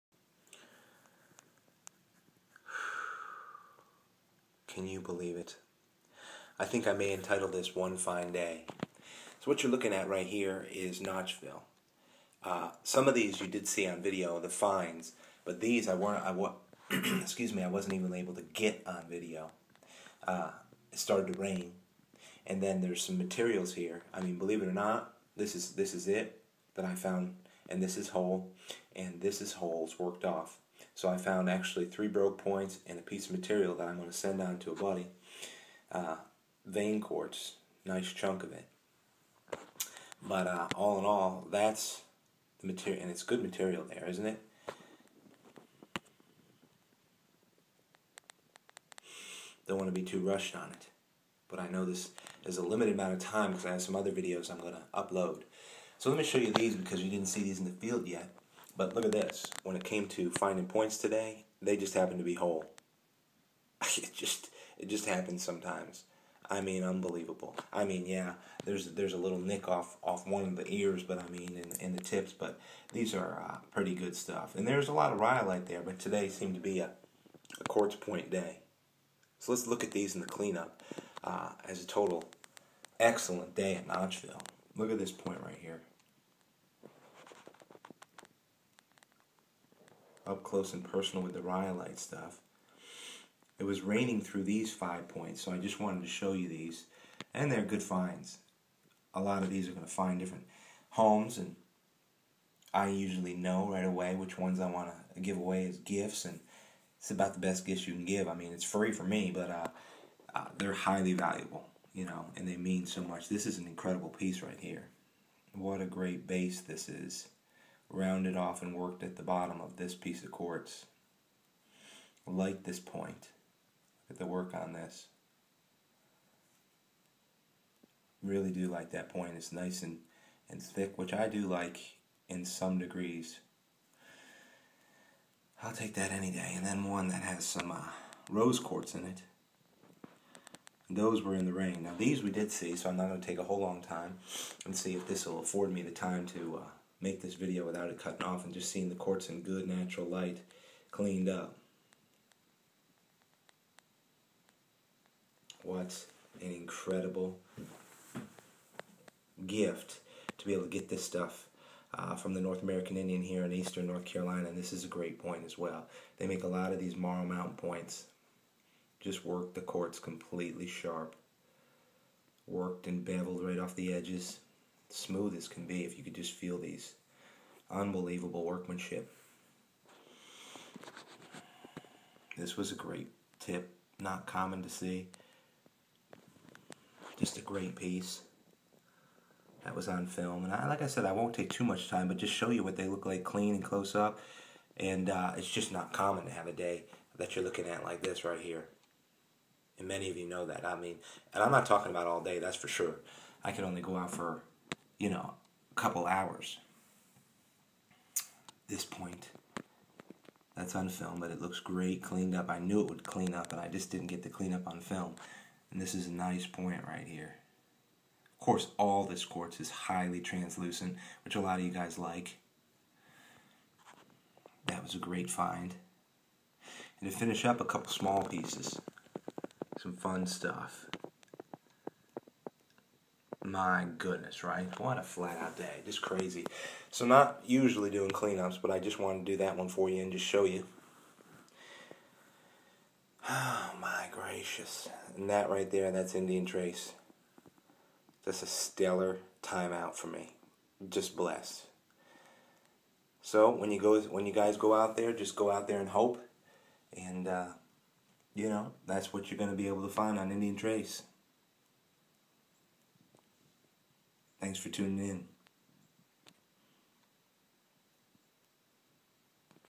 One Pound of QUARTZ Arrowheads! ~ Nov 2015
This video shares the results of 'one fine day' in the world of those who like to find & save North American Indian artifacts!  I should actually say, 'one fine time out'!  Work hard to find those sacred locations... then keep moving forward!